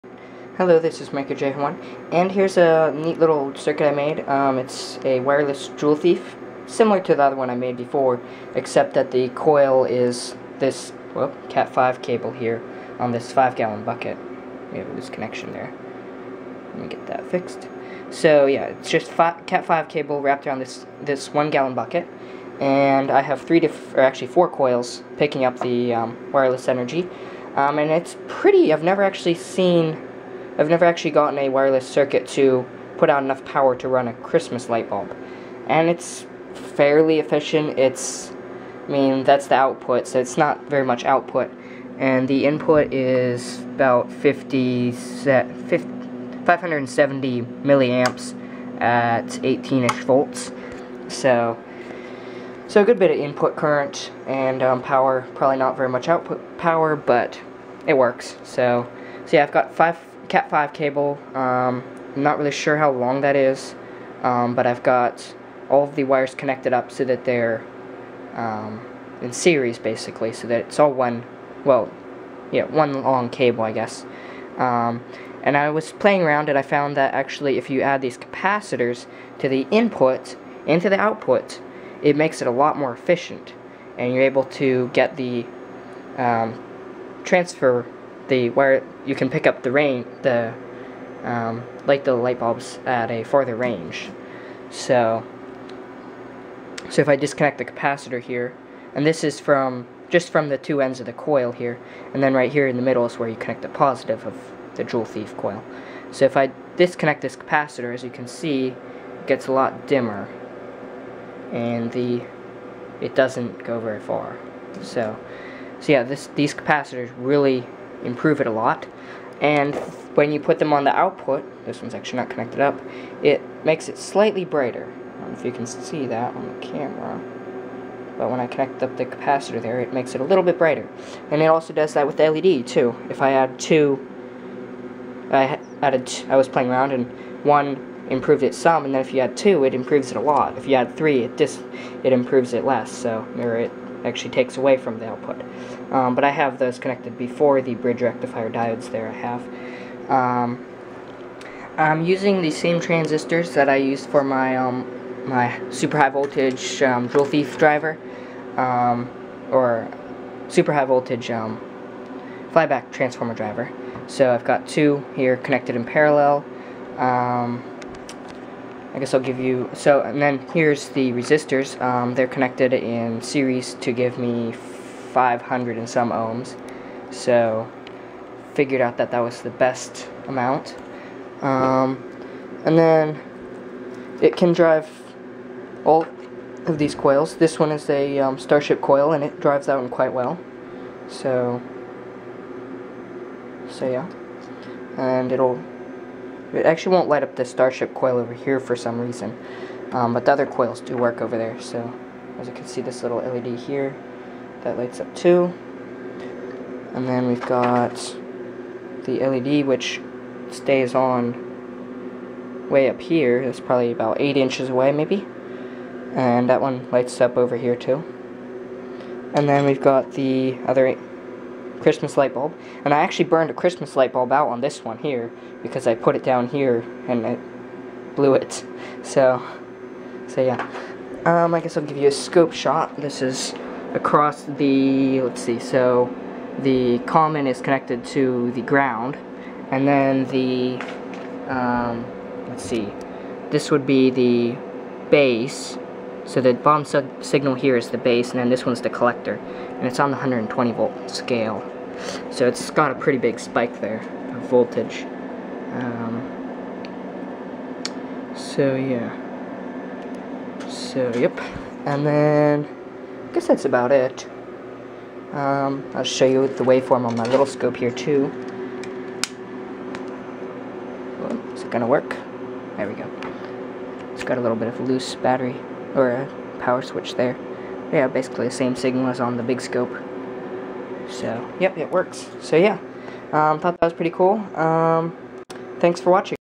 Wireless Joule Thief Using CAT5 Cable
I tried using a 2n3055 but it didn't work any better then these two 2SC5047 that I used for my super high voltage joule thief fly back transformer driver